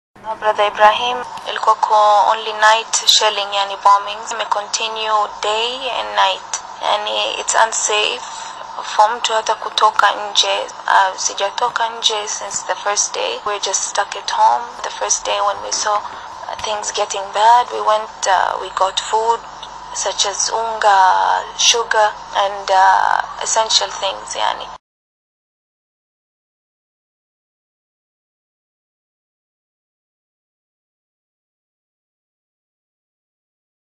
Kenyans cry for help from war-torn Yemen to return back home
More than 3,000 KENYAN nationals are stranded in Yemen and have appealed to the government to help evacuate them and bring them back to the country.

Right now  they are unable to get any help from Kenya, as it does not have any mission there.

Kenyan Foreign ministry in Nairobi has not responded as other countries are trying to rescue their citizens from Yemen.

Houthi rebels in Yemen have recently seized the capital city and for the past days it has become dangerous to live in the capital city of Sanaa.

All Airports are closed down and there are no flights due to airstrikes every day and night, a Kenyan in Sanaa tells me when I called to find out the situation of Kenyans there.